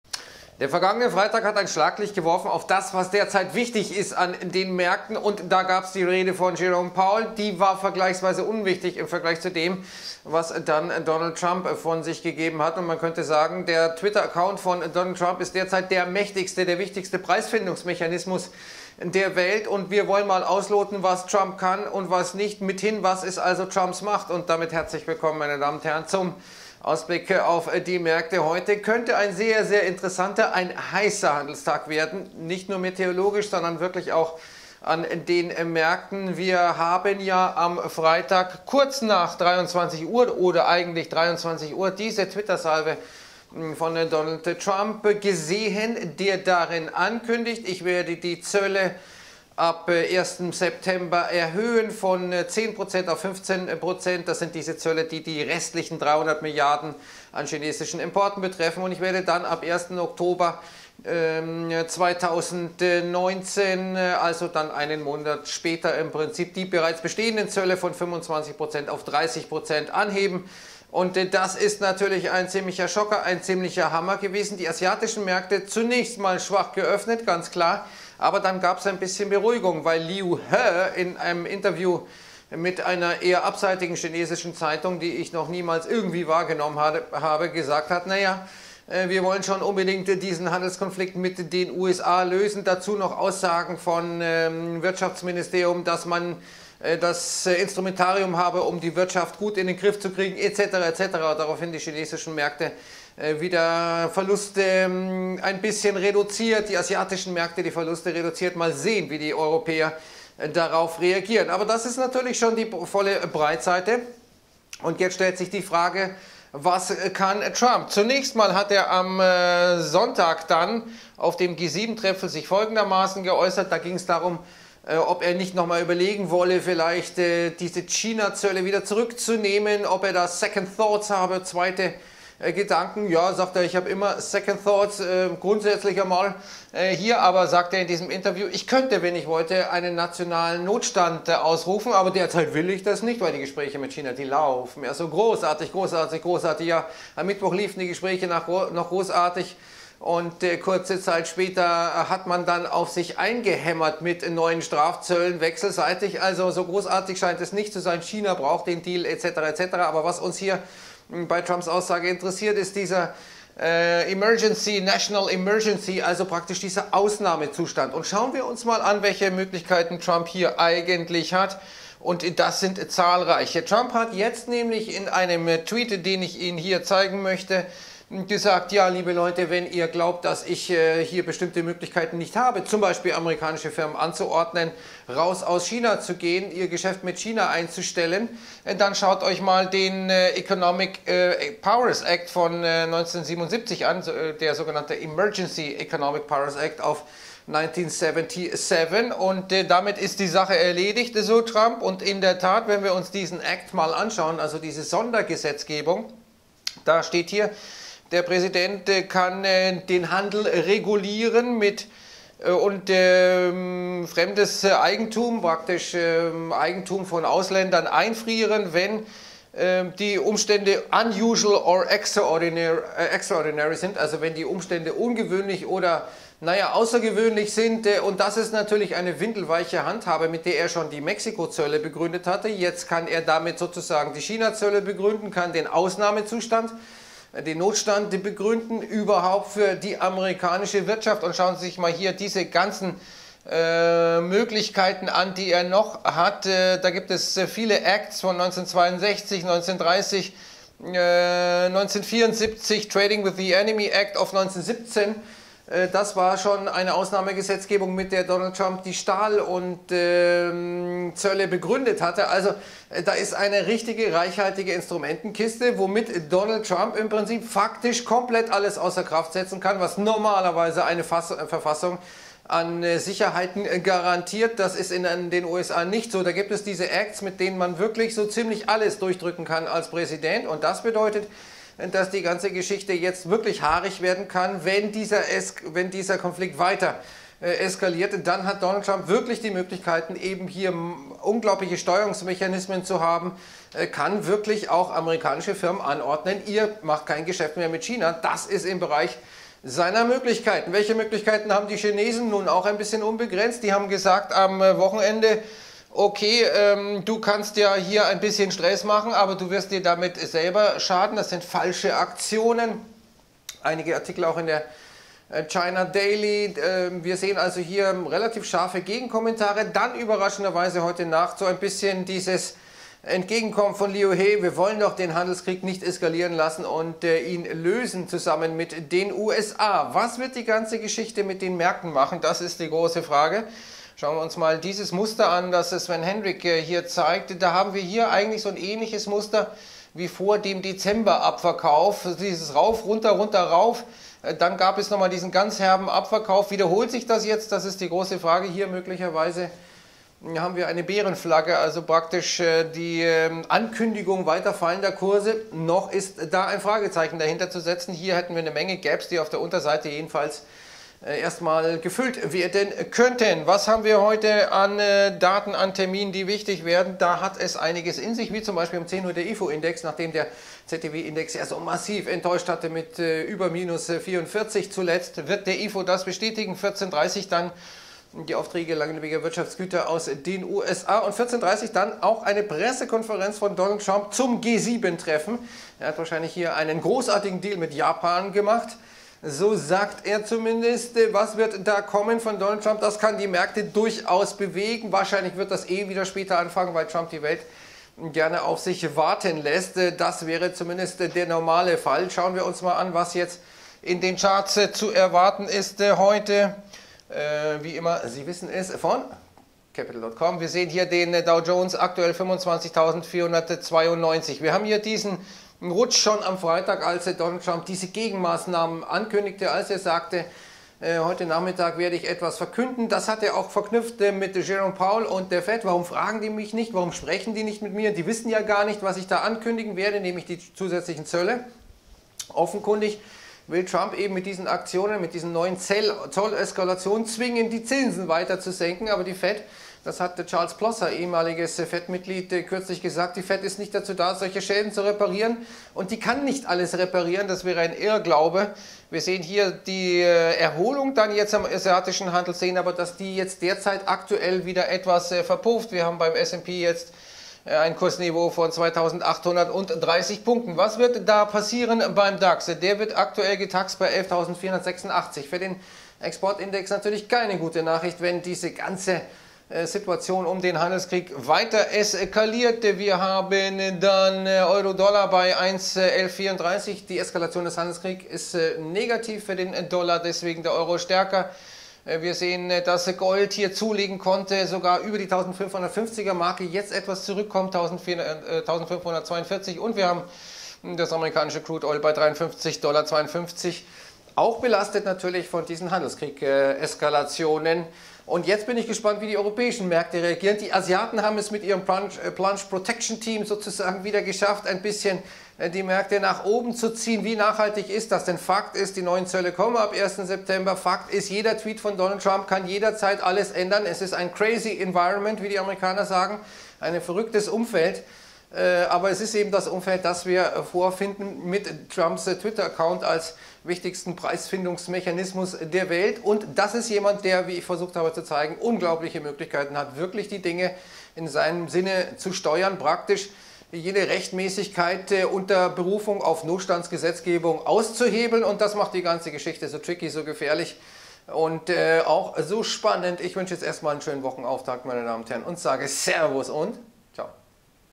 Trump - seine Macht! Videoausblick
Keine Frage: Trumps Twitter-Accout ist derzeit der mächtigste Markt-Beweger - so wie am Freitag, als der US-Präsident dann kurz nach Schließung der US-Futures (in Reaktion auf Chinas Gegenmaßnahmen) die Anhebung der Zölle gegen China bekannt gab. Die Märkte daraufhin mit stärkeren Verlusten, die im asiatischen Handel durch Aussagen aus China abgemildert wurden (Liu He: wollen Streit mit USA lösen) - wie reagieren Europas Märkte?. Aber der ökonomische Schaden ist bereits angerichtet (extrem unsicheres Umfeld), die neuen Zölle ab 01.Septmber kommen - und vielleicht noch mehr, denn Trump hat mit Berufung auf amerikanische Sondergesetzgebungen fast unbegrenzte Möglichkeiten der Machtausübung und könnte so etwa in der Tat amerikanische Firmen dazu zwingen, China zu verlassen..